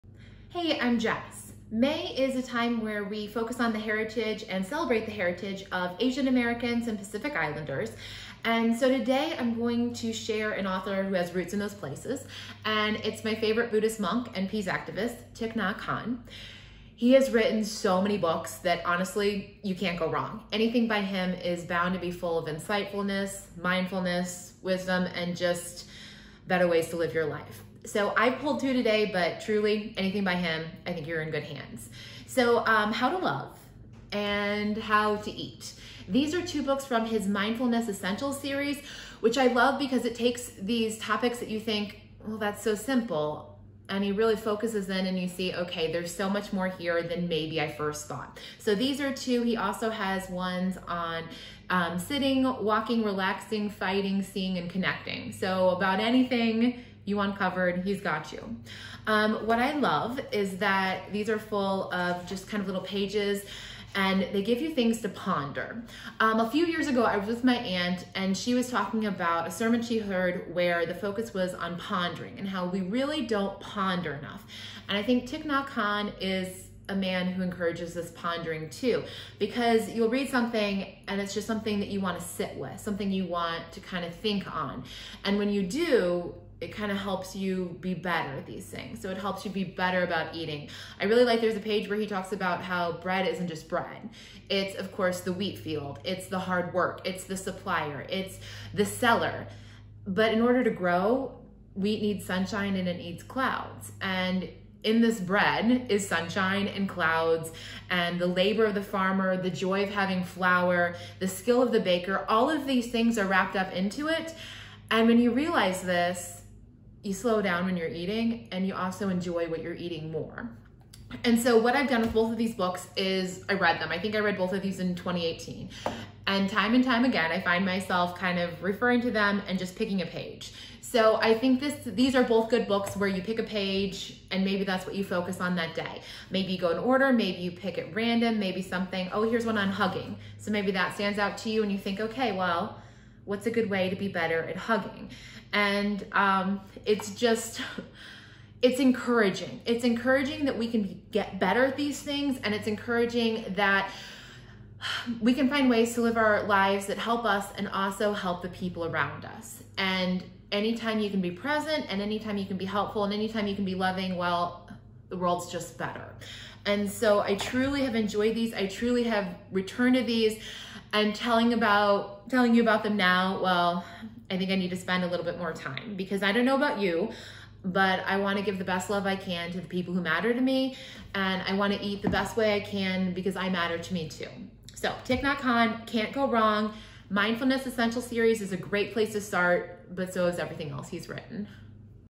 Mindfulness Essentials by Thich Nhat Hanh | Asian & Pacific Islander Heritage Month Book Suggestion
Mindfulness Essentials by Thich Nhat Hanh and illustrated by Jason DeAntonis
Asian & Pacific Islander Heritage Month Book Suggestion
I'm Jess. I'm a librarian and I only recommend books I've read and enjoyed.